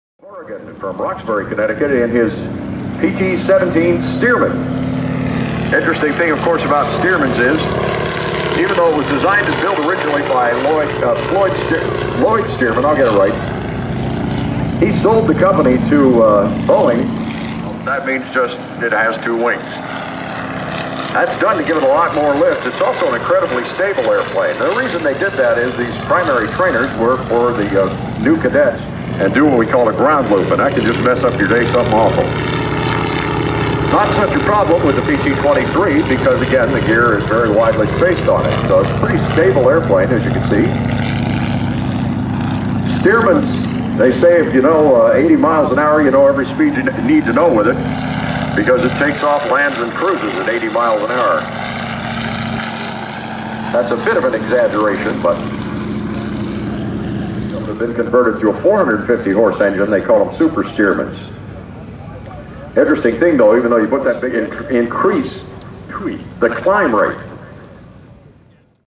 Westfield Intl. Airshow 2006 - PT-17 & PT-23
PT-17 and PT-23 flybys at the 2006 Westfield International Air Show at Barnes Municiple Airport located in Westfield, MA.  June 25th, 2006.